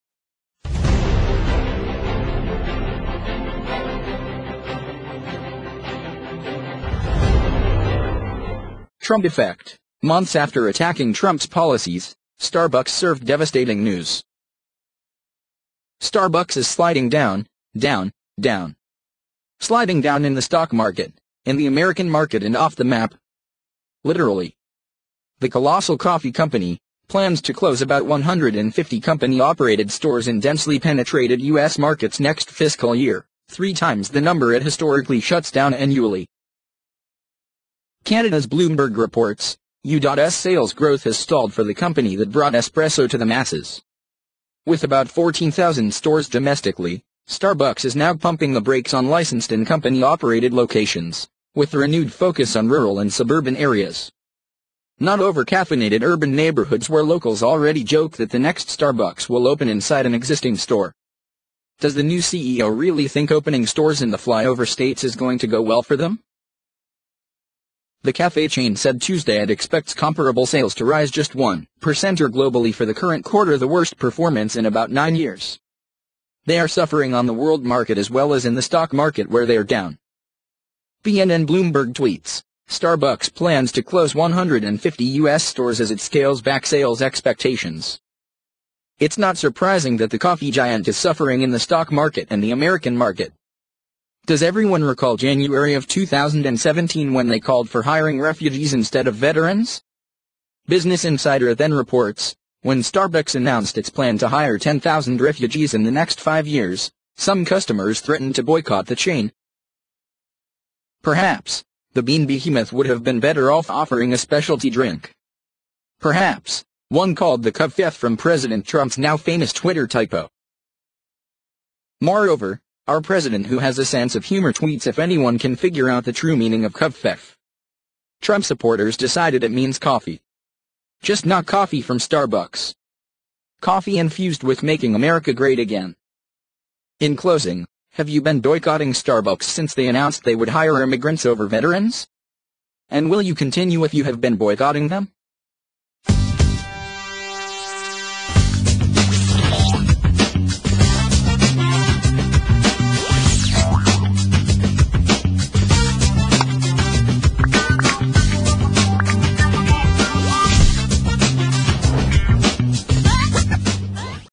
Months After Attacking President Trumps Policies Starbucks Served Devastating News 🔥 (GM MW)
This video, "Months After Attacking President Trump’s Policies, Starbucks Served Devastating News", was made by USA BREAKING NEWS and was provided to A Billion Views via creative commons.

USA BREAKING NEWS' comments below